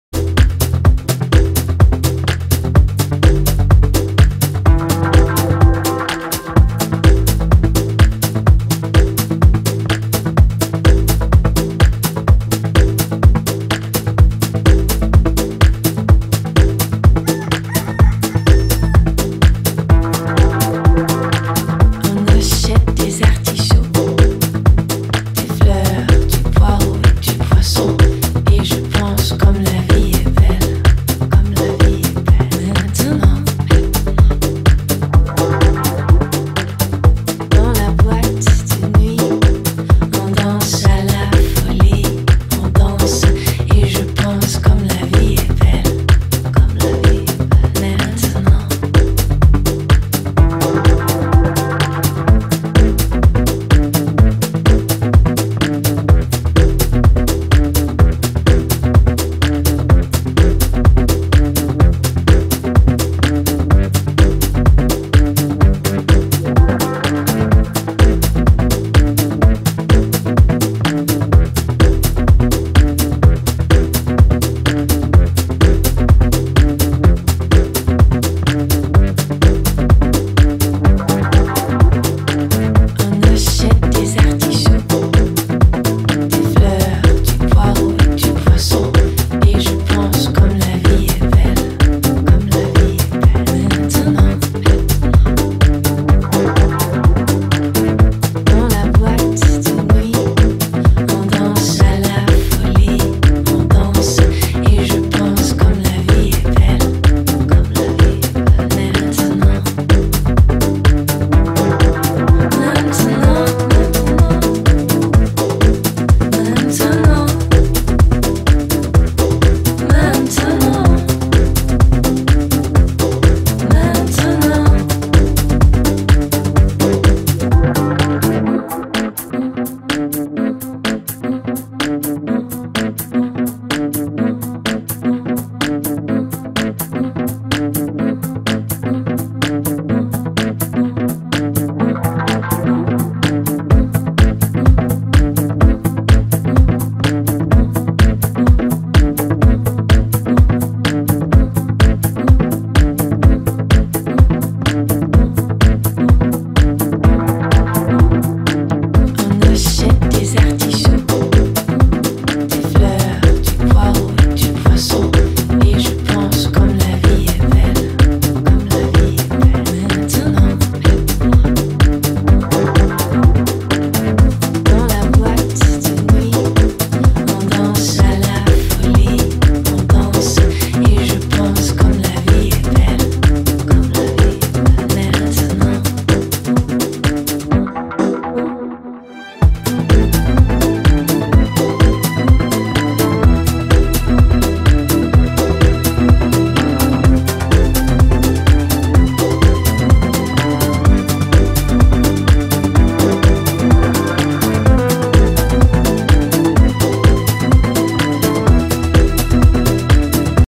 Смешные КОШКИ Funny CatsMy red cat
Мой Луи на окне занимается скалолазаньем, чистит ковер и спит со Стратосом.
Наши КОШКИ, КОТЫ и Котята Смешные Животные для Детей Funny Cats and Kittens CHILDREN HOME VIDEO

                                                                                          Много сердечек❤️